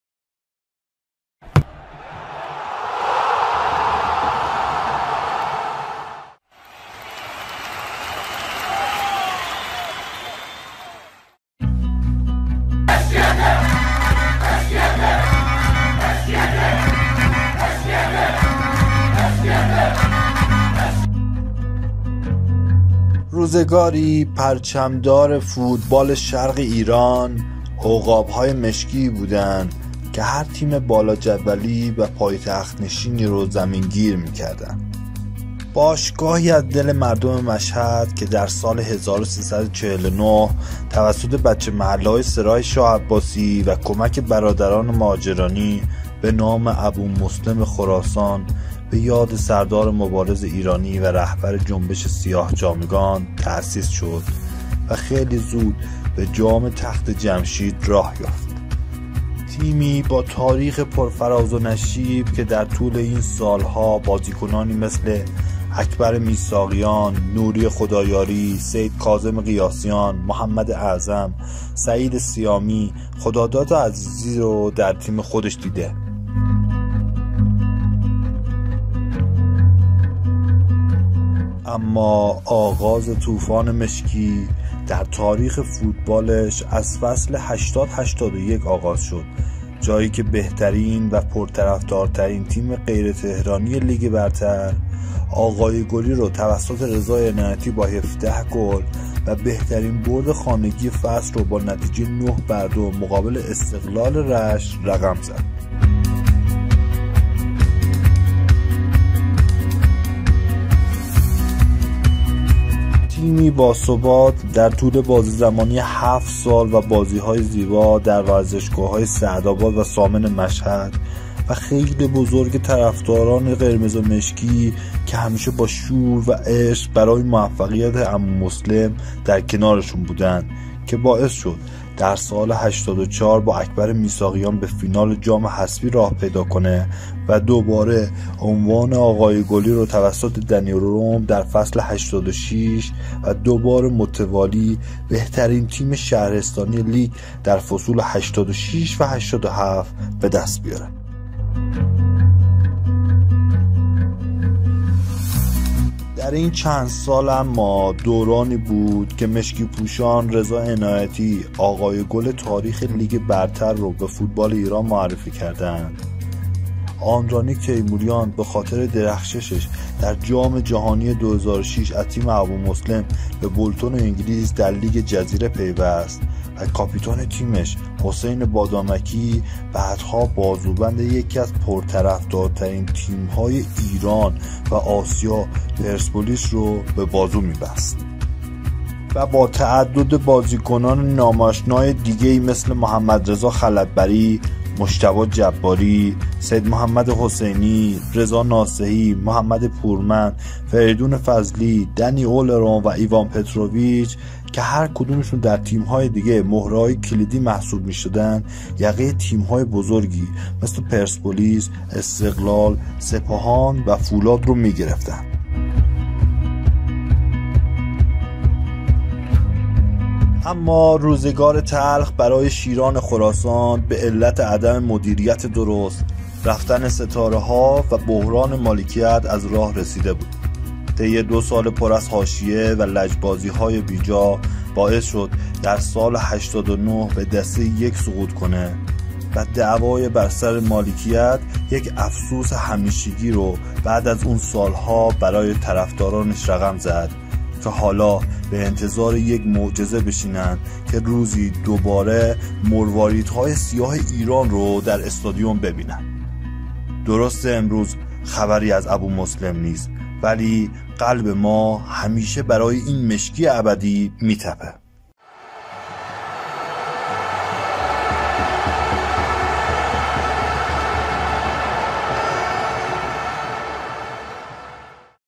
Aboomoslem FC - ابومسلم
F.C. Aboomoslem
Football Club Aboomoslem Khorasan commonly known as Aboomoslem, is an Iranian football club based in Mashhad, Razavi Khorasan, that competes in the League 3. The club was founded in 1970 and is named after Persian general of Abbasid Era, Abu Muslim, who was commander of the Abbasid Revolution and led the Abbasid Revolution that toppled the Umayyad dynasty.

ابومسلم خراسان
ام این باشگاه برگرفته از فرمانده نظامی ایرانی حکومت عباسیان است که حکومت بنی امیه را سرنگون کرد. آخرین حضور فوتبالی این باشگاه، به پاییز سال ۱۳۹۲ بر می‌گردد. در حال حاضر، تیم فوتبال ابومسلم خراسان به دلیل عدم حمایت، در مسابقات فوتبال ایران حضور ندارد. البته به گفته مدیران این باشگاه قرار است دوباره در سال 1400 به لیگ دسته دو ایران بازگردد .

Narrator & Written : Saeed Monfared
Edit : Saeide Saatchi & Saeed Monfared
Collector : Saeide Saatchi & Shahram Erfanian
نویسنده و راوی : سعید منفرد
ادیت : سعیده ساعتچی - سعید منفرد
گردآوری : سعیده ساعتچی - شهرام عرفانیان